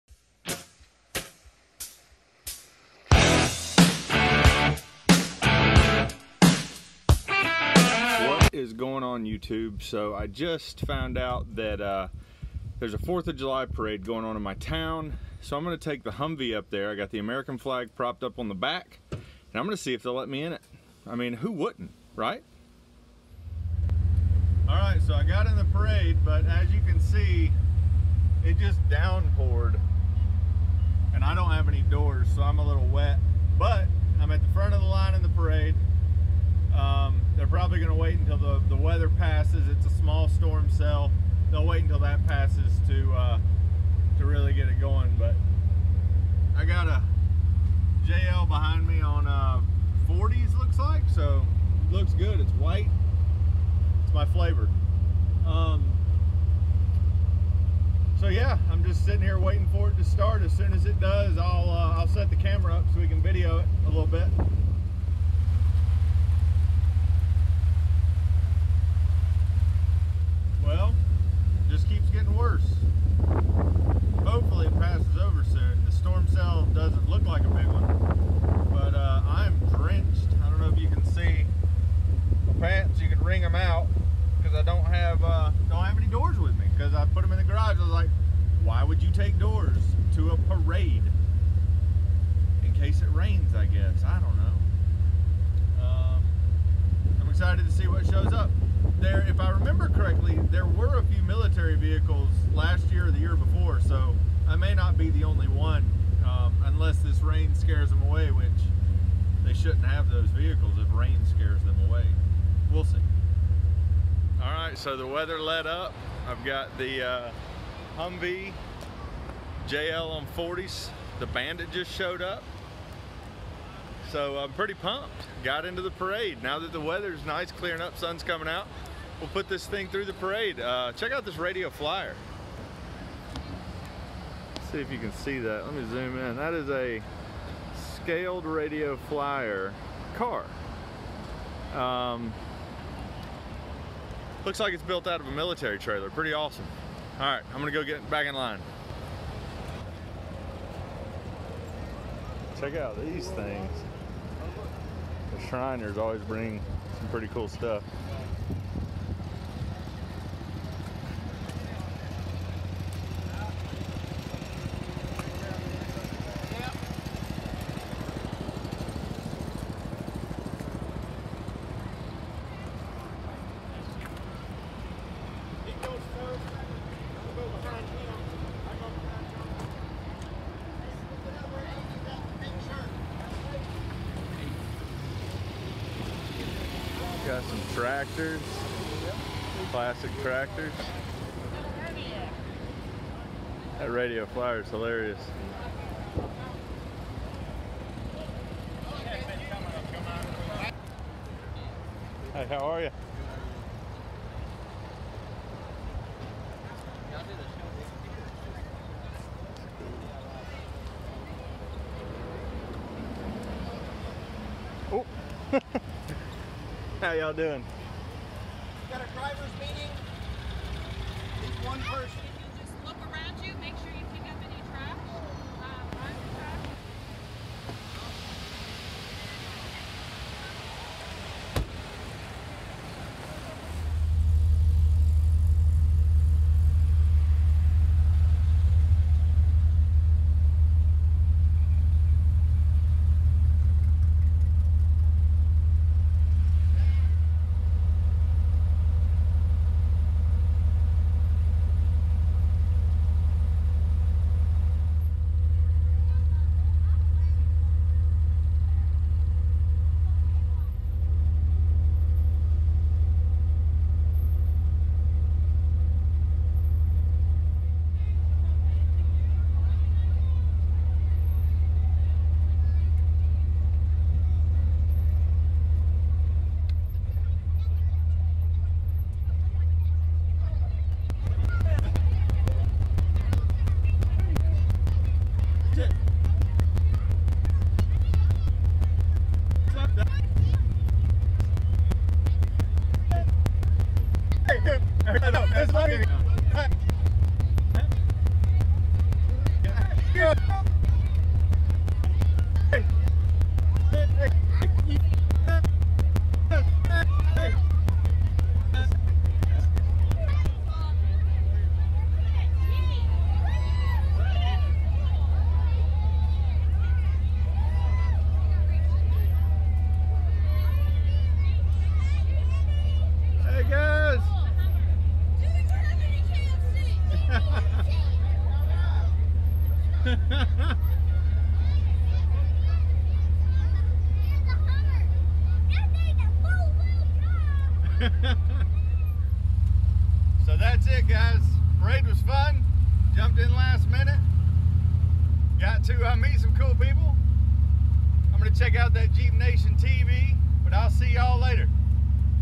I snuck into a 4th of July Parade and got SOAKED! - LSxHumvee
I heard of a little parade going through main street where I live so I figured I'd take the Humvee over and see if they would let me tag along. Well they did but I ended up getting soaked right before we started. It was a good time and there ended up being a ton of people there.